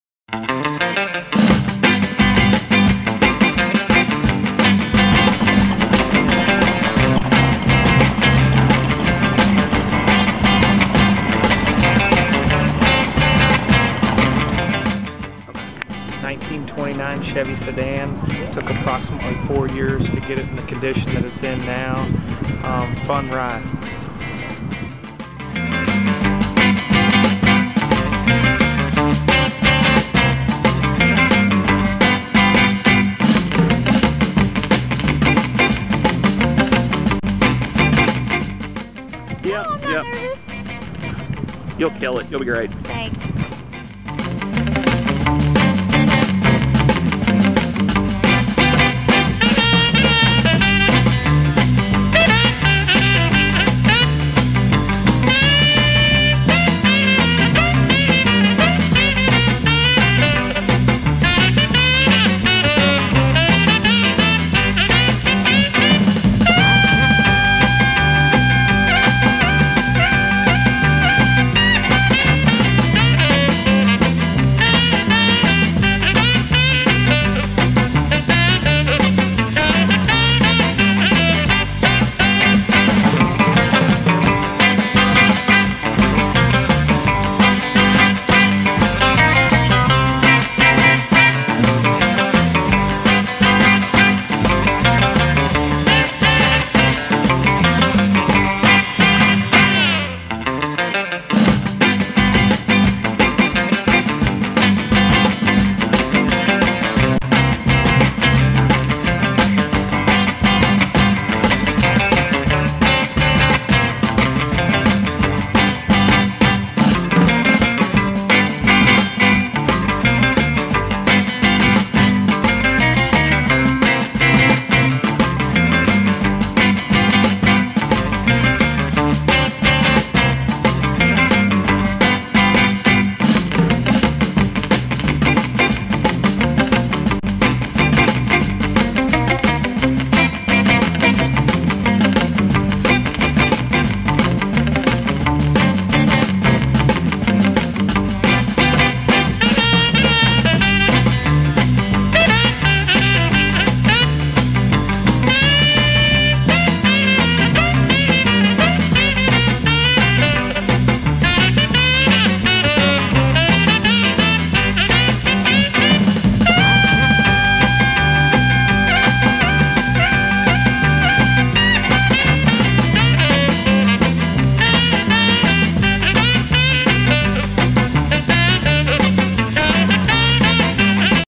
'29 Chevy, A Pinup Girl and a Hobo House - Behind the scenes with Boudoir Louisville
Go behind the scenes with Boudoir Louisville as he shoots pics of pinup model Kellie with an awesome '29 Chevy Sedan!